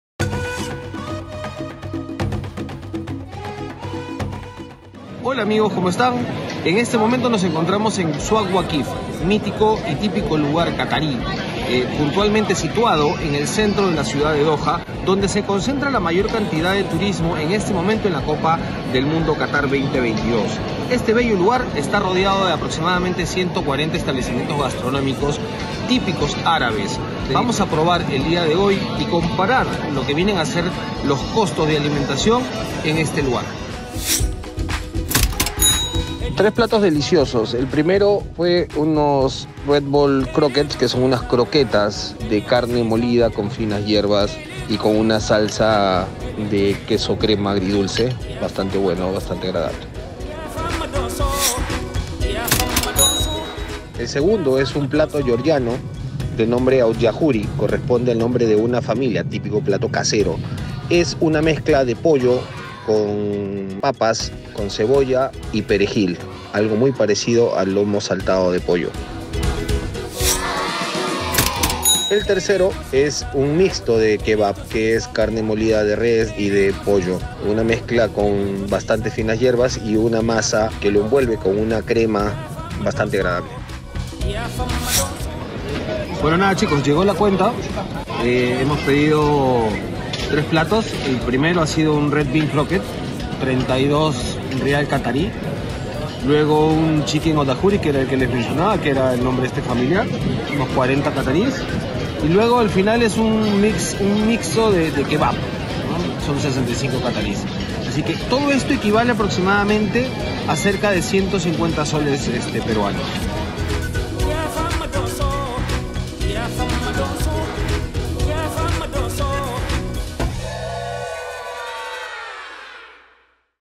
QATAR 2022 ¿Cuánto cuesta COMER en QATAR? Pedimos comida típica y así nos costó
¿Eso es lomo saltado y papa rellena? 😮😂 Probamos la comida típica de Qatar para contarte cómo es, a qué sabe, qué lleva y sobre todo, cuánto cuesta 🤑 ¿Realmente es caro comer allá? Mira el video y dinos, ¿tú hubieses pagado ese monto por solo tres platos? Te leemos 👇